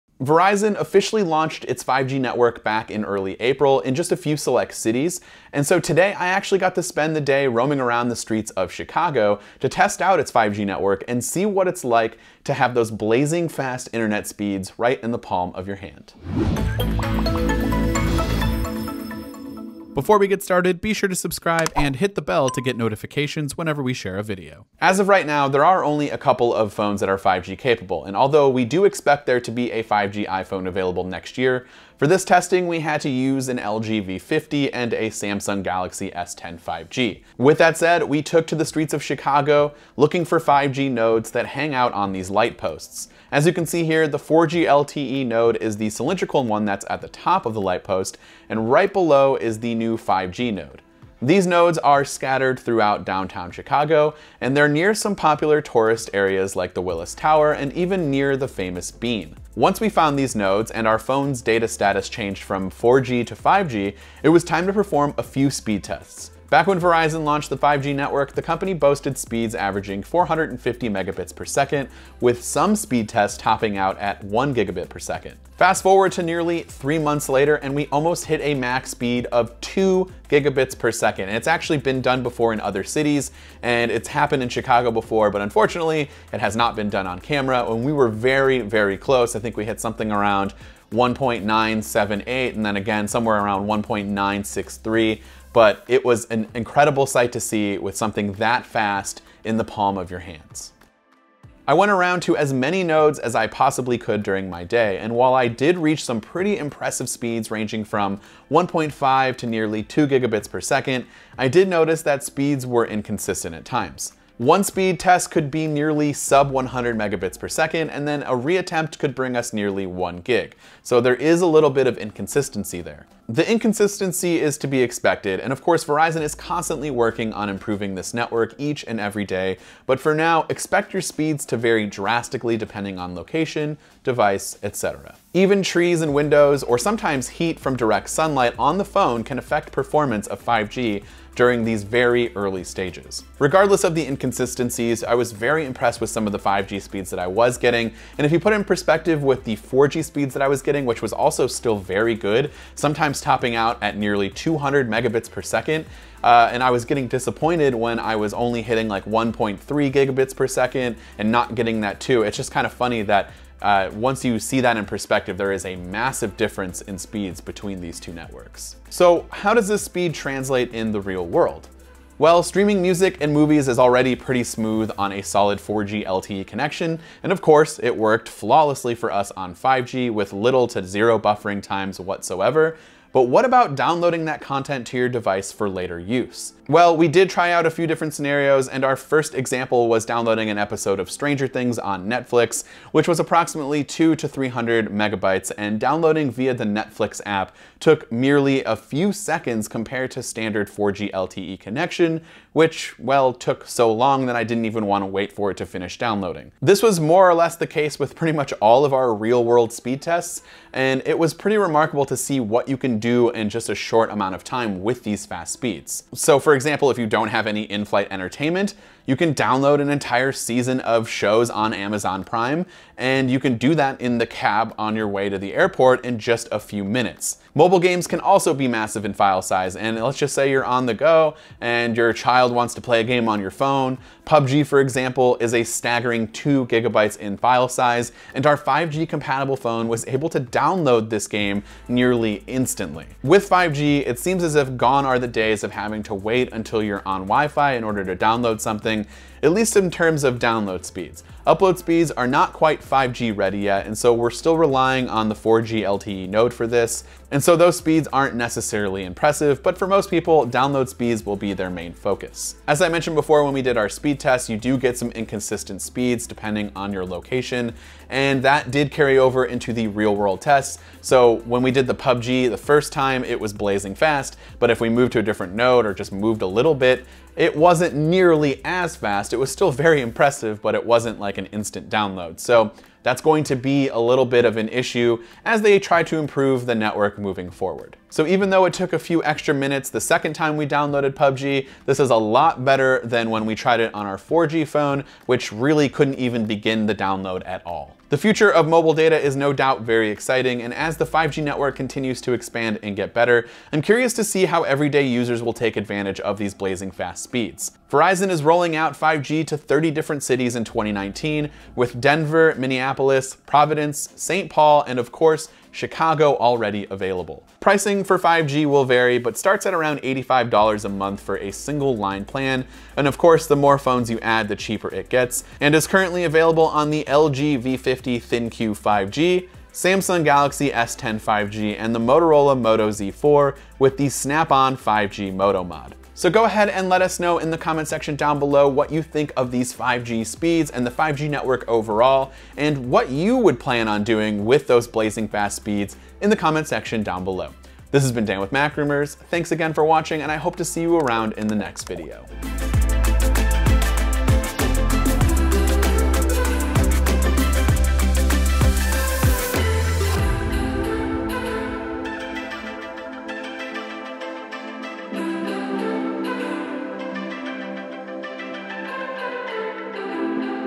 Testing Verizon's Ultra Fast 5G Network!
Apple isn't planning to launch a 5G iPhone until 2020, but carriers in the United States and other smartphone manufacturers like Samsung are already testing 5G, which offers download speeds that are much faster than 4G.

Verizon invited MacRumors to Chicago to test its 5G network, giving us an idea of what 5G speeds will be like when we can use 5G networks on our iPhones in 2020.